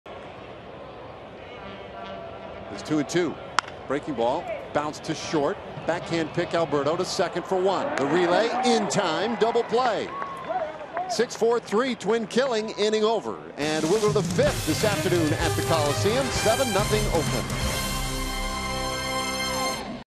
TEX@OAK: Alberto, Odor turn a double play in the 4th
Hanser Alberto makes the stop on a grounder and throws to Rougned Odor to start a 6-4-3 double play in the 4th inning